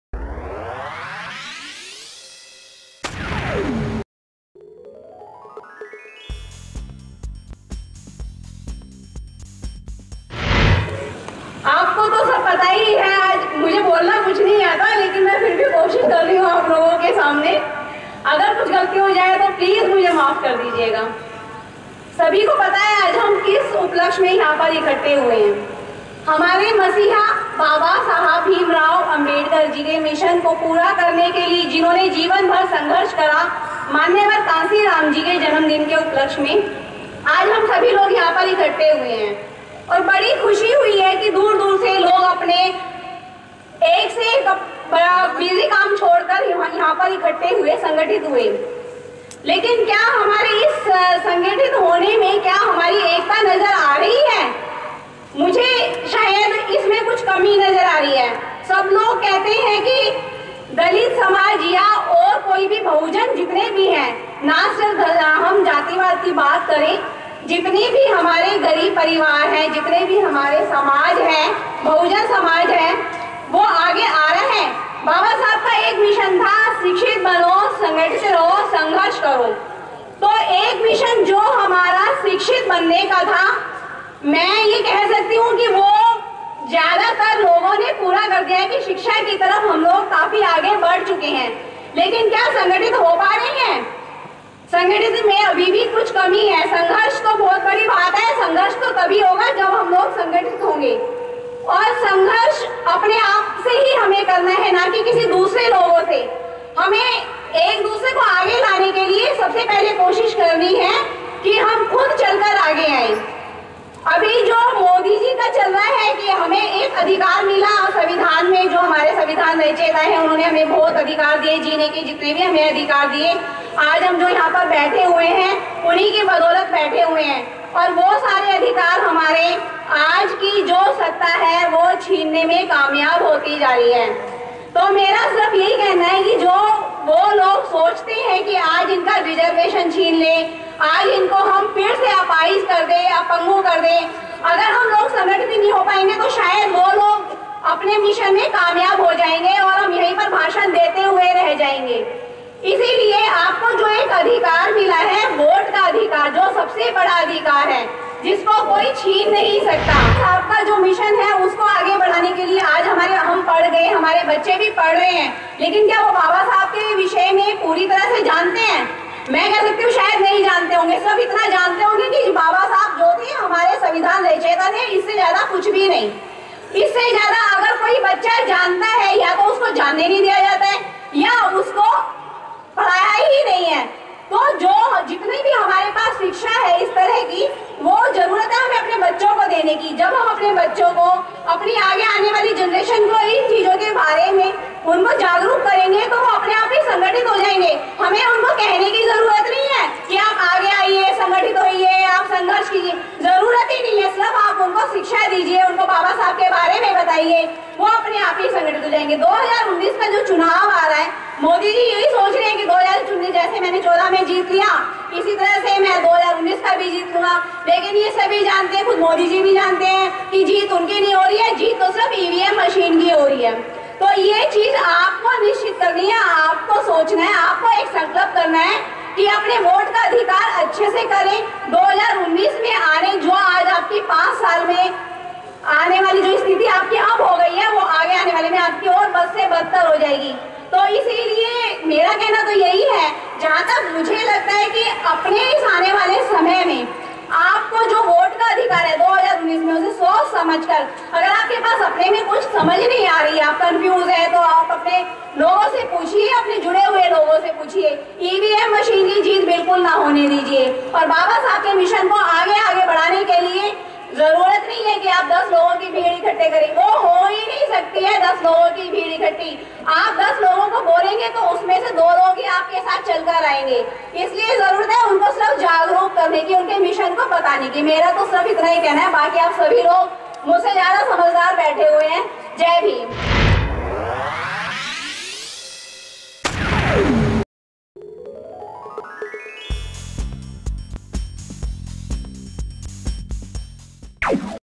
आज हमारे बच्चों को baba saheb के बारे में बताना चाहिए-Bahujan Sankalp Diwas-SM News
आज हमारे बच्चों को baba saheb के बारे में बताना चाहिए